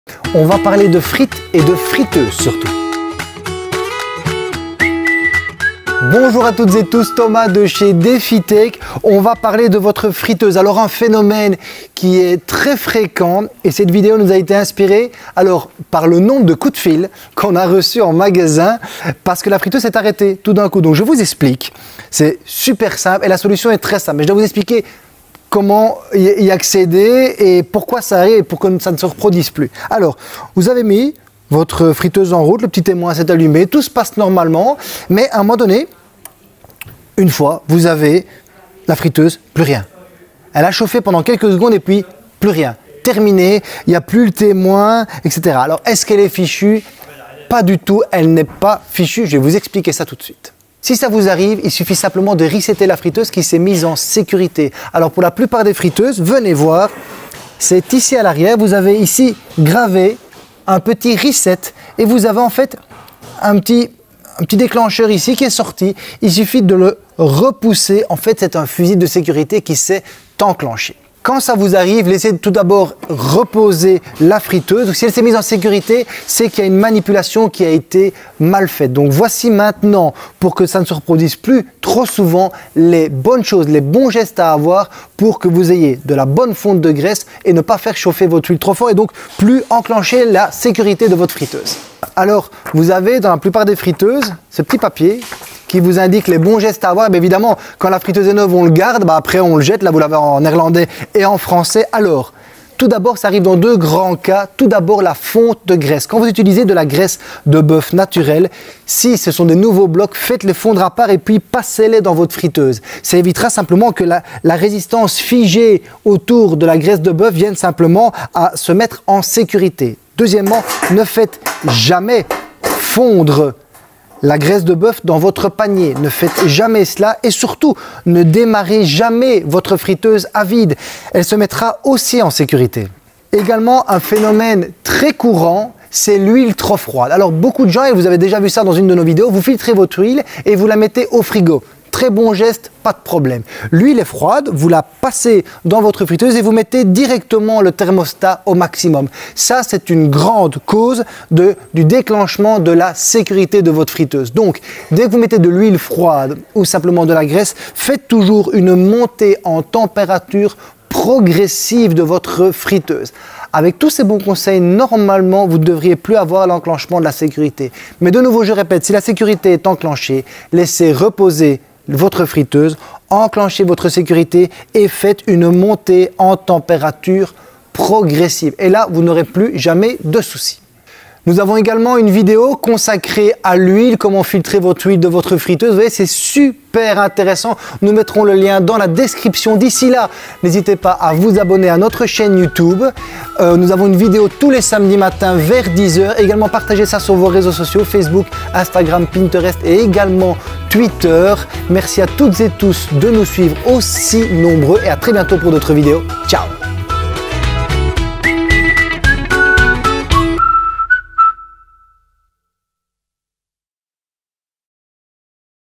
Votre friteuse est en panne ? Avez-vous enclenché le bouton reset ?
Si votre friteuse s'est arrêtée et qu'elle ne s'allume plus : elle n'est pas forcément foutue ! Laissez refroidir votre appareil au moins 30 minutes et enclenchez le bouton RESET à l’arrière de votre friteuse.

Ce phénomène de protection arrive dans 2 grands cas : La fonte des blocs de graisses et de l’huile trop froide.

Pour éviter cela, suivez mes conseils dans cette vidéo ou sur notre blog :

Pour commander nos friteuses sur notre site de vente en ligne

A bientôt,
Thomas